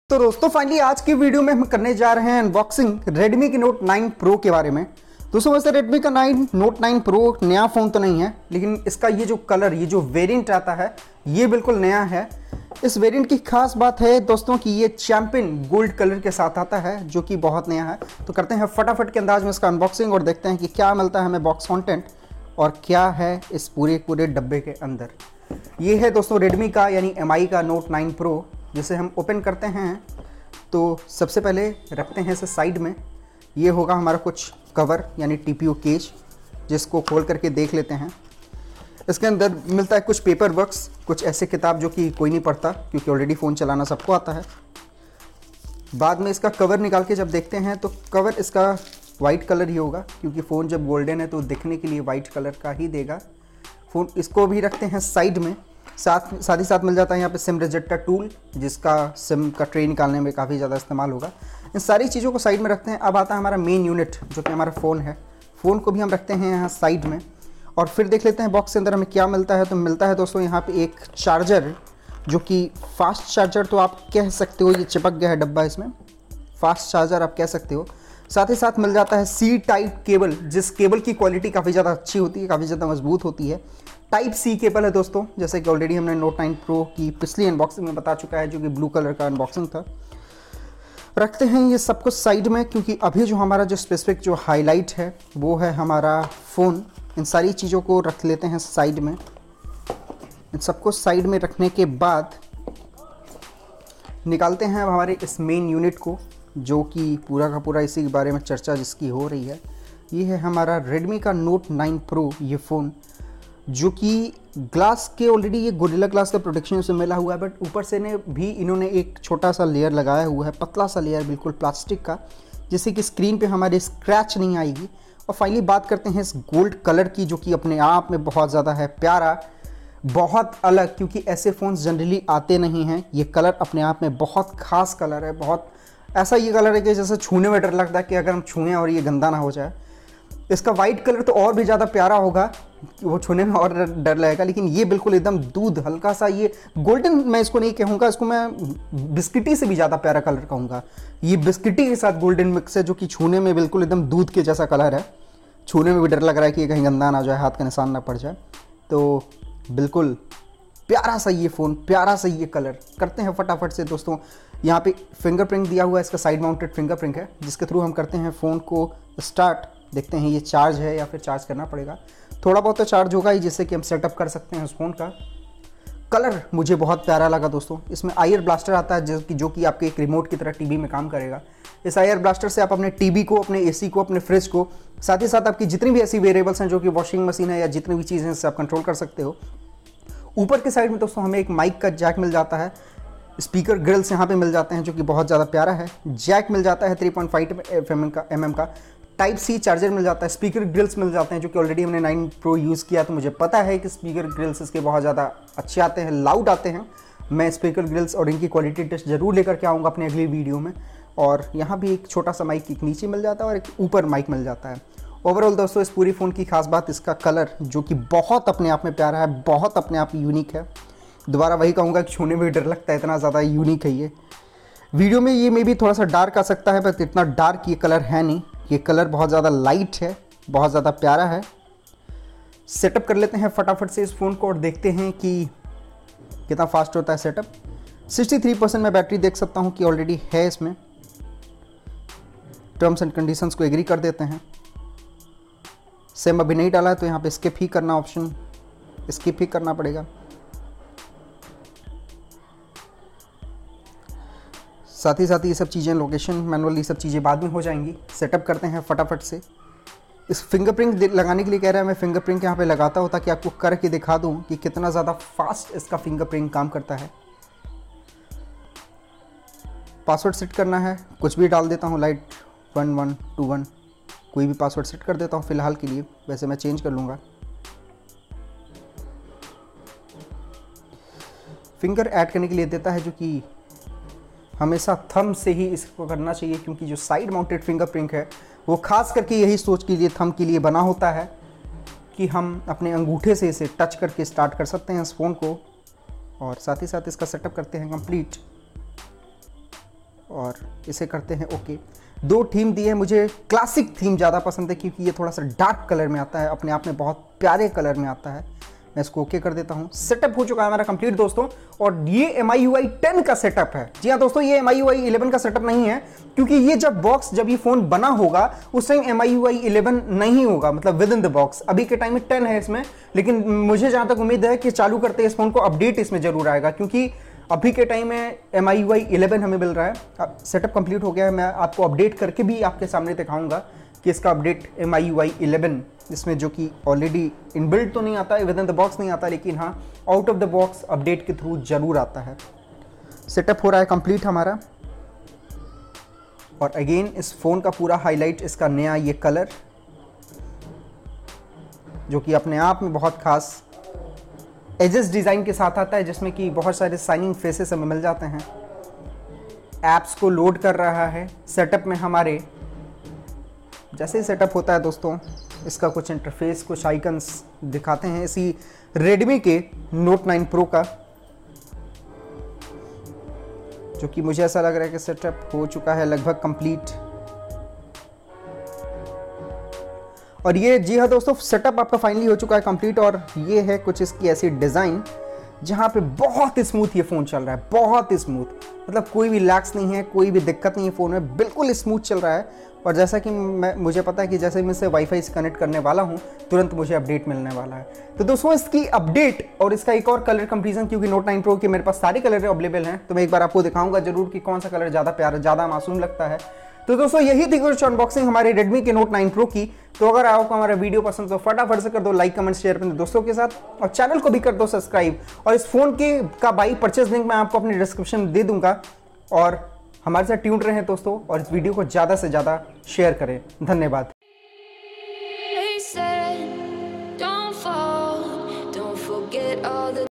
REDMI NOTE 9 PRO - UNBOXING & FIRST LOOK REVIEW | CHAMPAGNE GOLD | TechnoticsWorld
About : TechnoticsWorld is a YouTube Channel, where you will find technological videos in Hindi, New Video is Posted Everyday :)